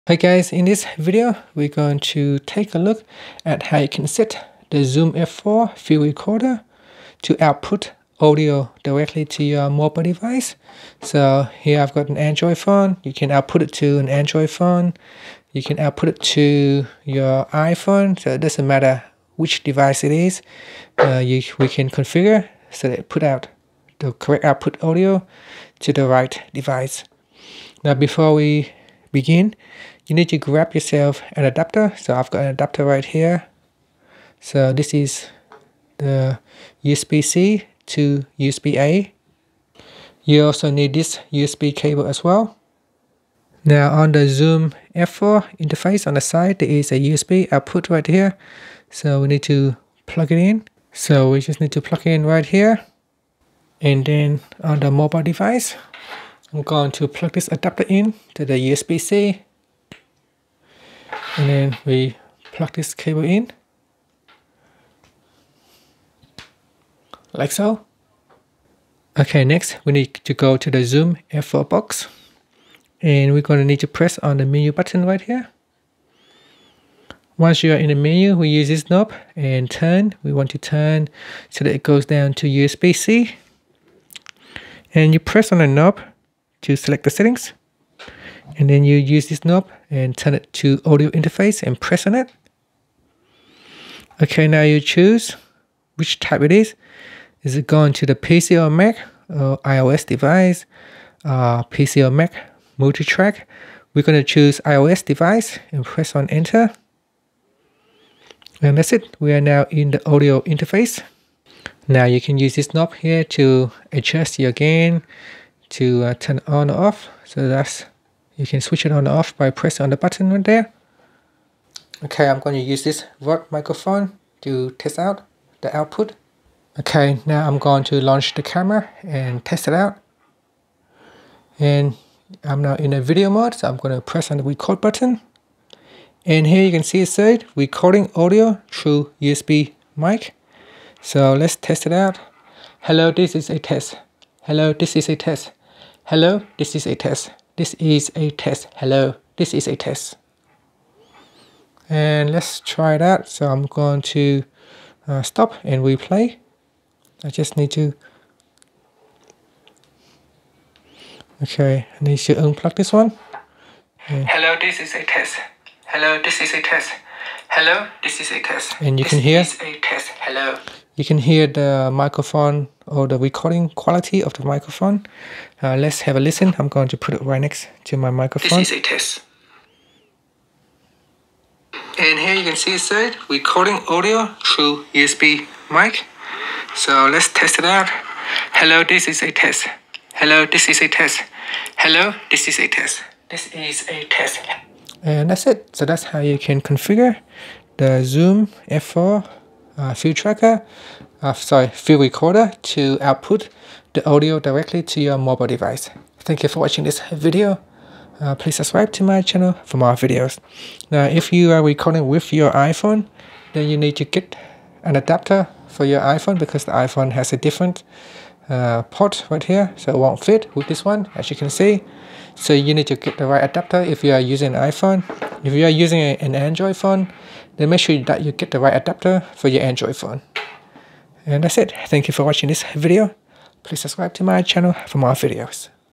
How to Set Zoom F4 Field Recorder to Output Audio Directly To Android Phone/iPhone/ iPad / Galaxy S
Learn how you can set Zoom F4 Field Recorder to output audio directly to Android Phone / iPhone / iPad or Samsung Galaxy Phone.